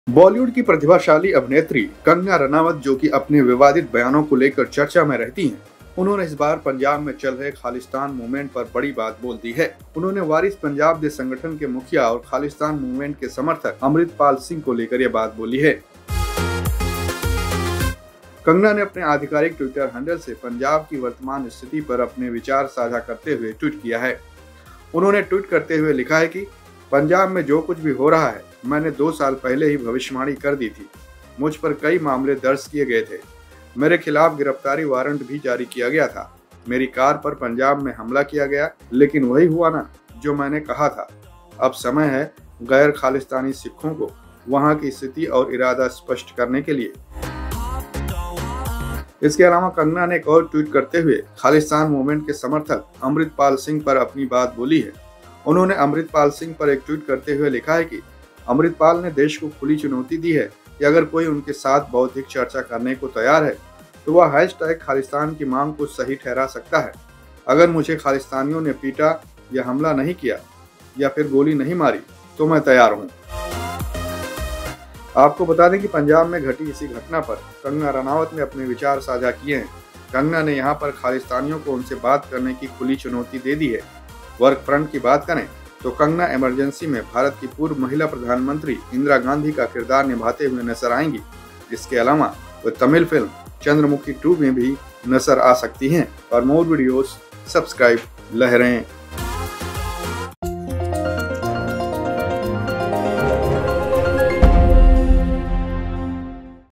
Kangana Ranaut said this on Khalistan Movement Amritpal Singh
बॉलीवुड की अभिनेत्री कंगना रनौत ने खालिस्तान मूवमेंट के समर्थक अमृतपाल सिंह पर एक बड़ा बयान दिया है।